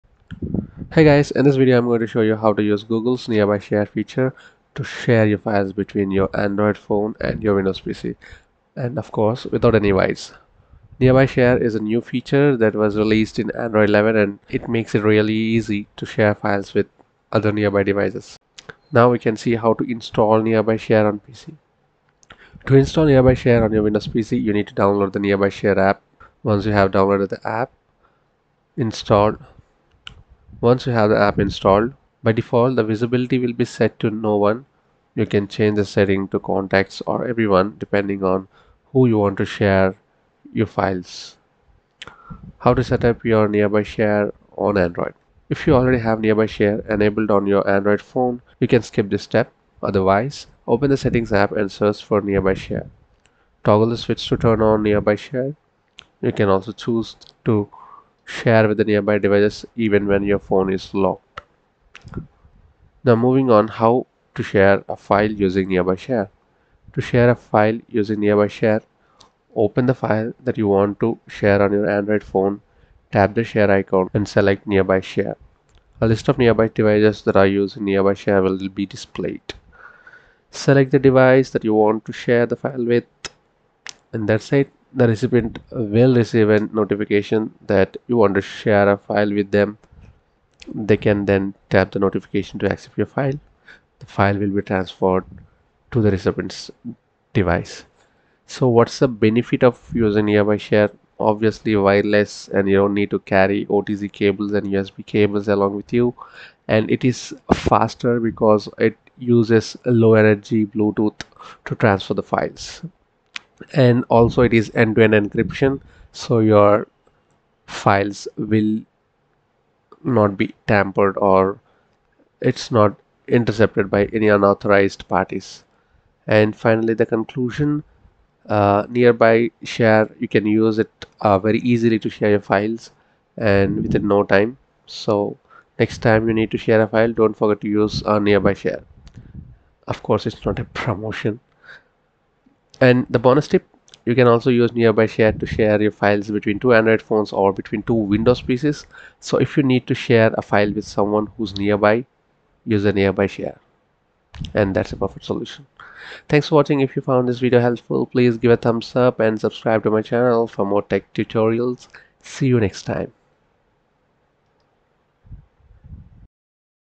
How to transfer files from mobile to pc easily | How to use nearby share in windows
With Nearby Share, quickly transfer photos, videos, documents, audio files or entire folders between your Android device and Windows desktops and laptops.